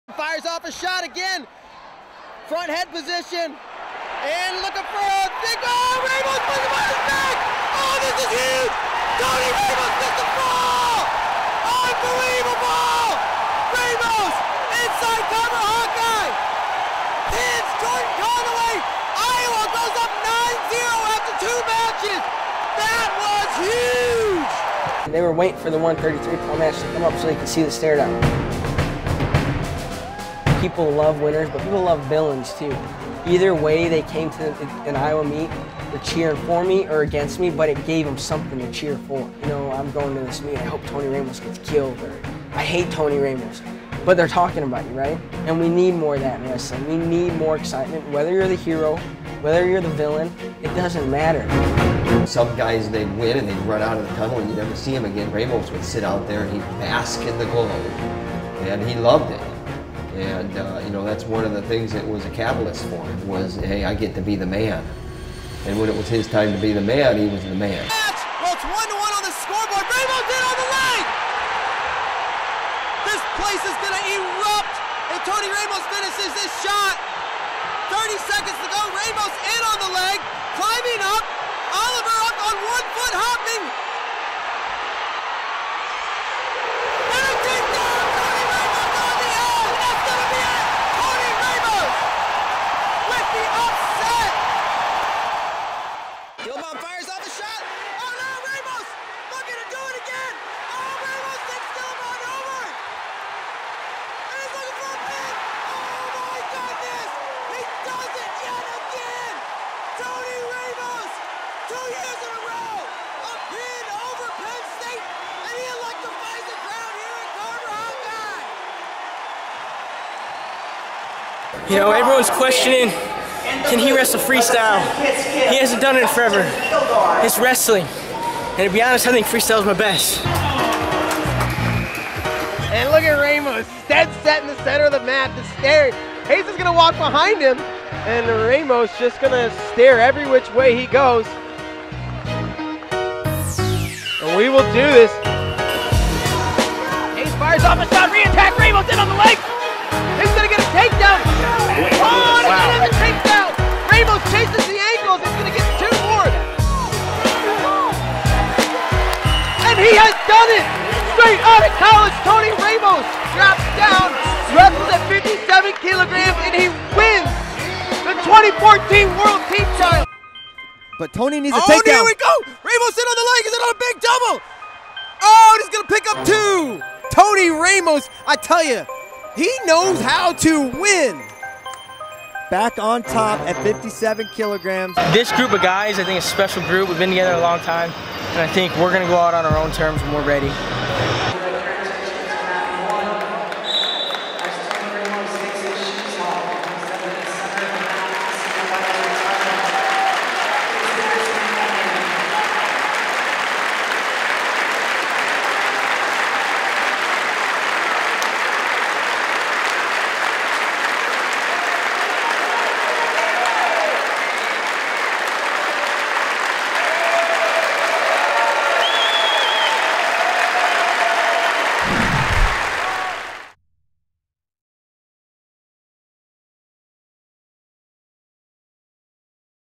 Tony Ramos Career Tribute
Check out some of the best moments in Tony Ramos collegiate and international career.